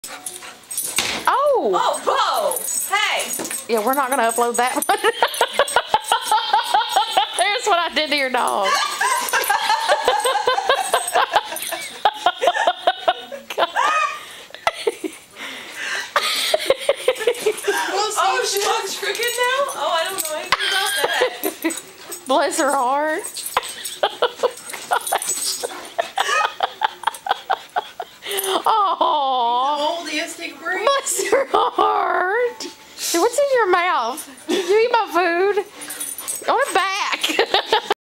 Emmie and Bocephus Rumble
The blow sounds a lot worse than it was!